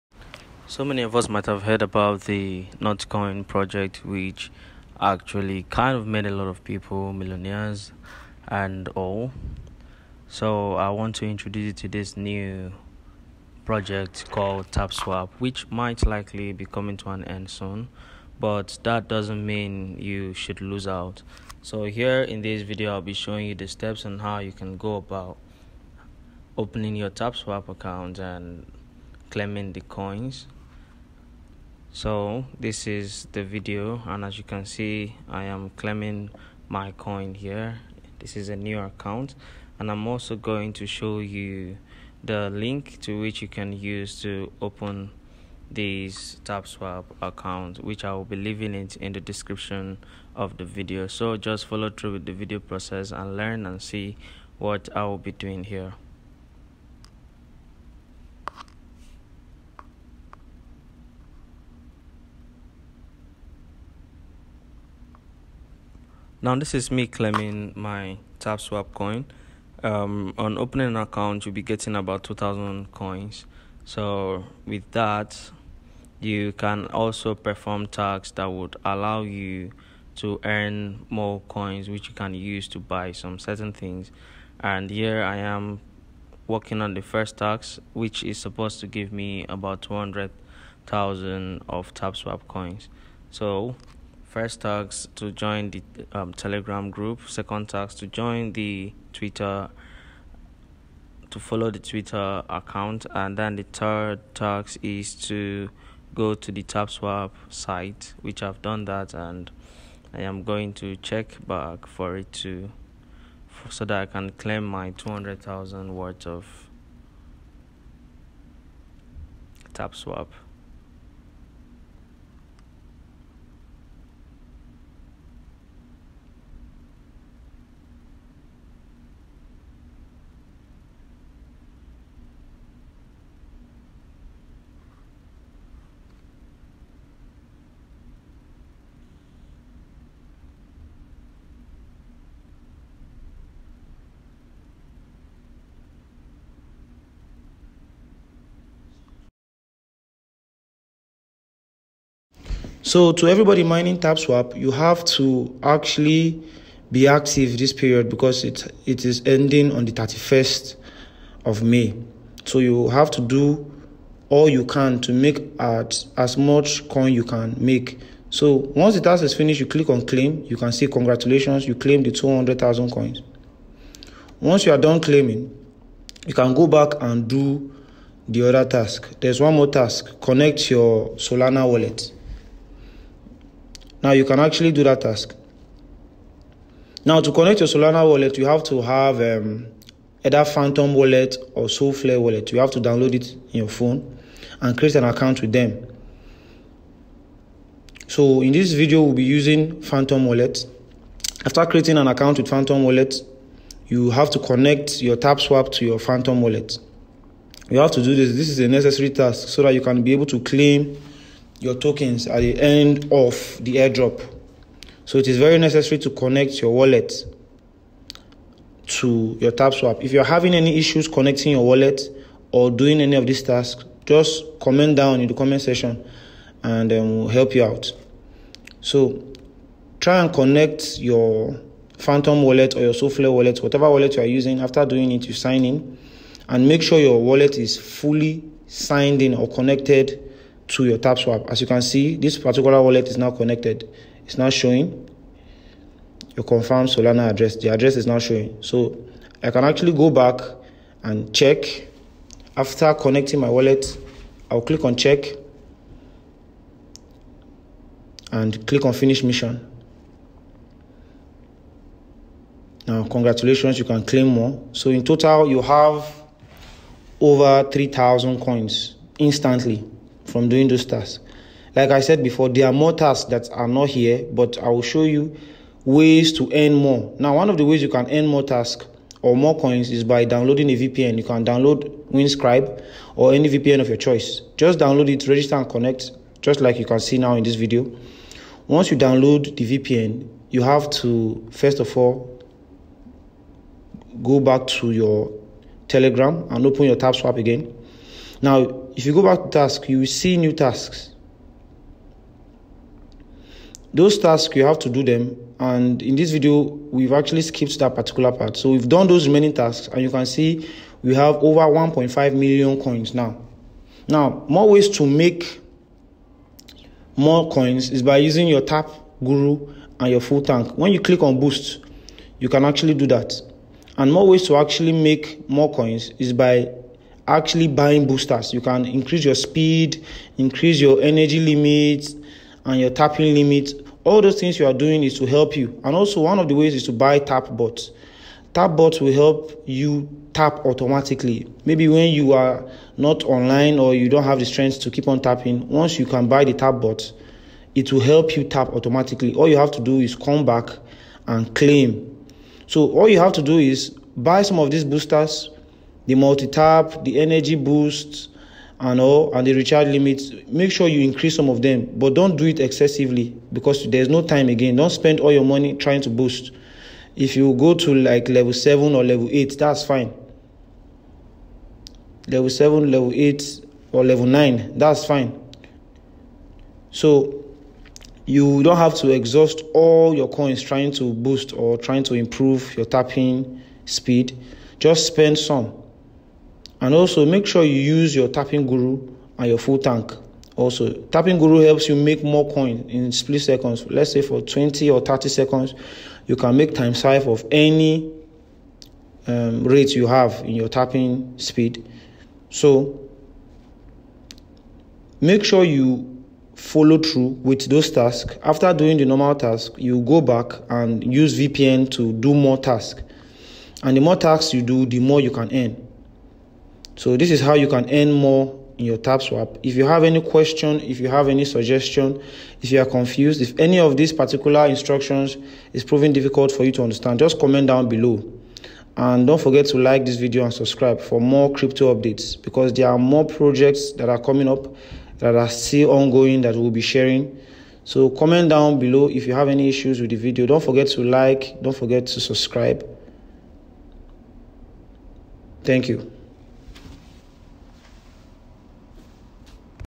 New Update on TAPSWAP | How to Connect the Solana Wallet on TapSwap Step-by-Step Guide
In this video you will Learn How to Connect the Solana Wallet on TapSwap Telegram Bot Mining Step-by-Step Guide. Ensure Not to Skip any part of the video so you will learn how to do it correctly..